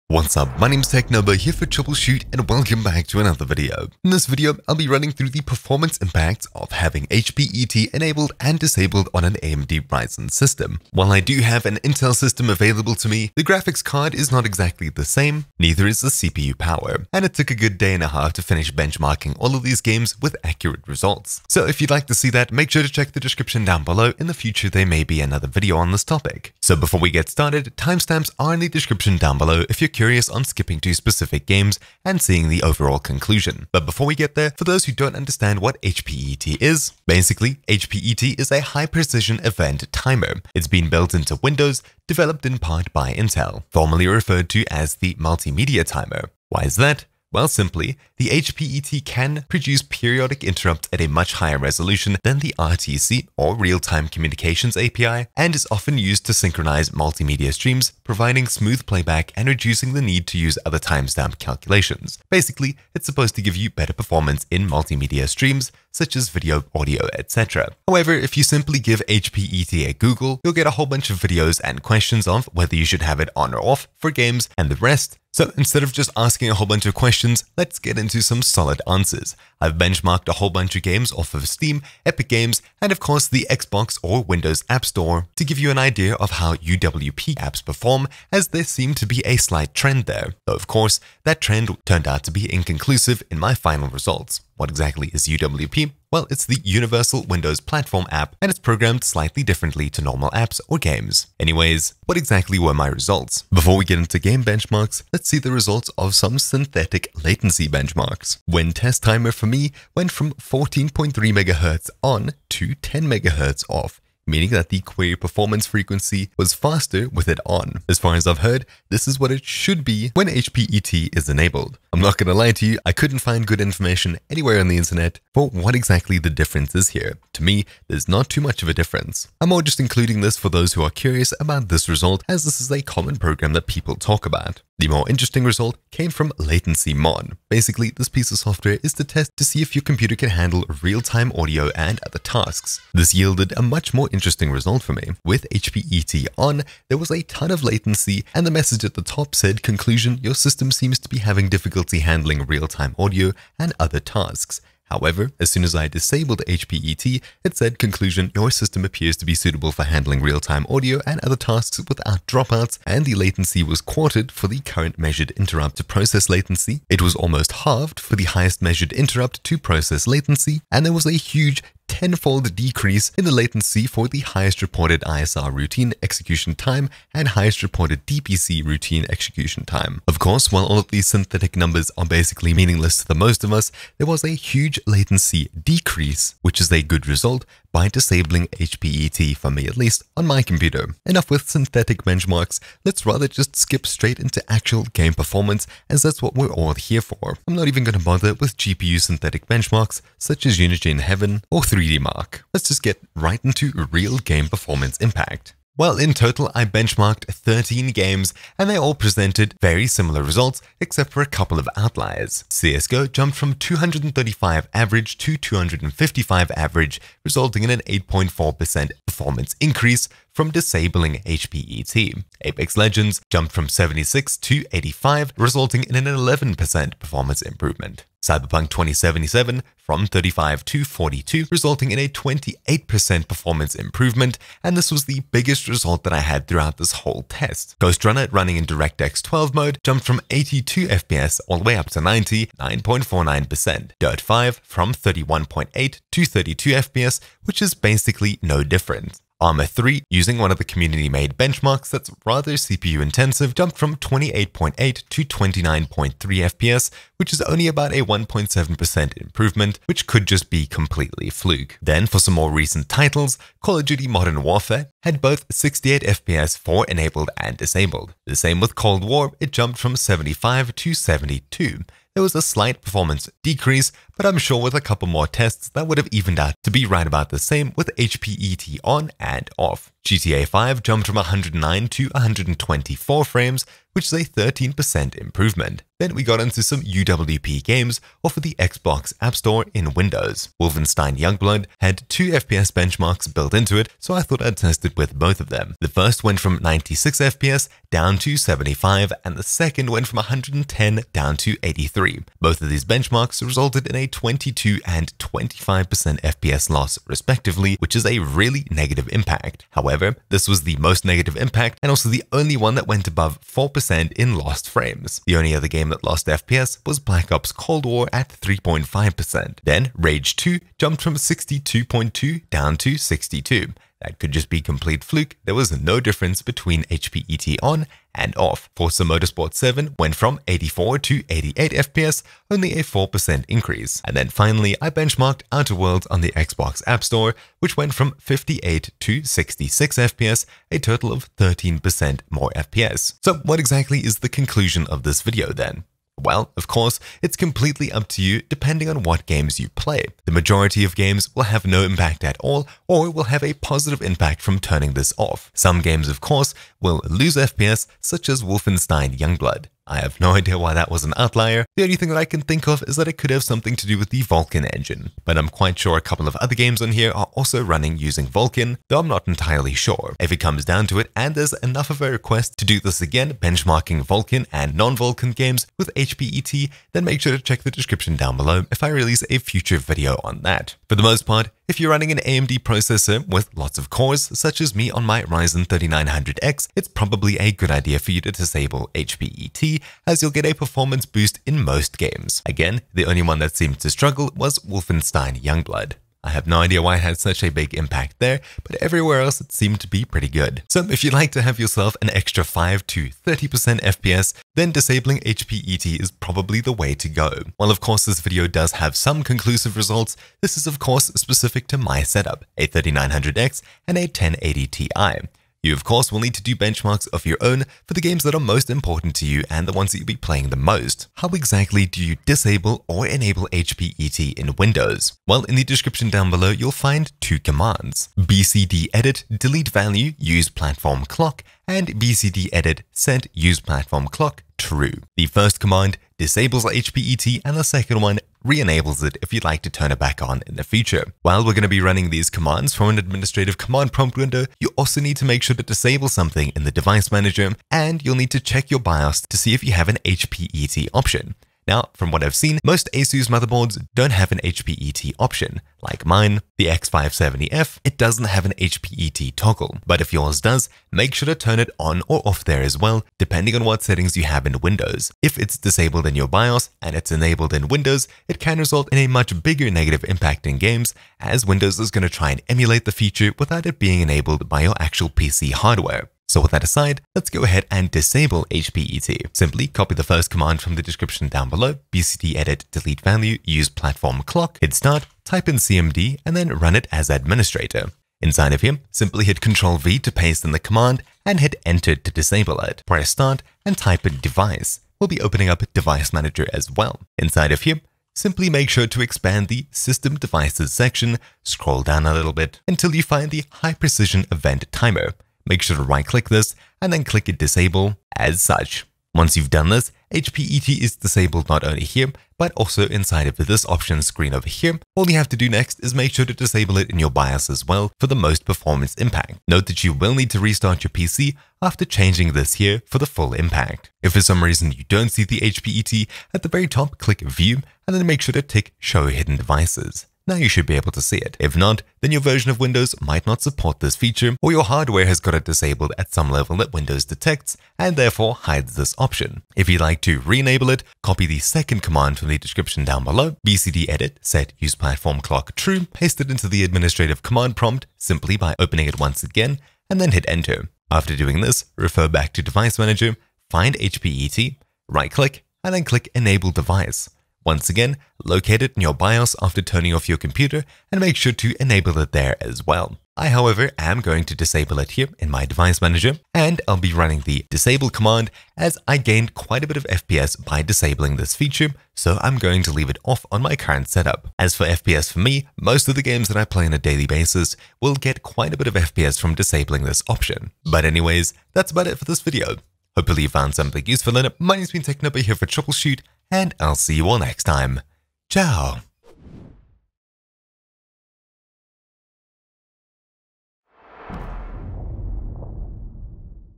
AMD: HPET On or Off? | Off = Less latency & stuttering, More FPS in MOST GAMES!
Heard about HPET? Should you disable HPET? Will you get more performance? Well, I benchmarked a couple of titles and compared the differences on my AMD Ryzen 3900X and 1080 Ti. While some games were GPU limited, and others CPU, there was still an improvement for most of them with HPET being set to...

Disable HPET: bcdedit /deletevalue useplatformclock
Enable  HPET: bcdedit /set useplatformclock true

Timestamps:
0:00 - Explanation
0:34 - What is HPET?
1:42 - Latency changes
3:25 - Game Benchmarks
5:43 - Conclusion. Should you turn it off?
7:00 - IMPORTANT NOTE on turning HPET Off & On
7:56 - How to: Turn off HPET
8:43 - Find missing HPET option
8:59 - How to: Turn on HPET